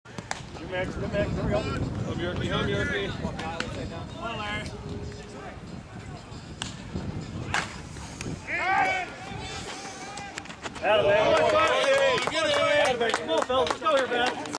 20110514115746.m2ts
USSSA Conference Tournament in Columbus, Indiana May 13-14, 2011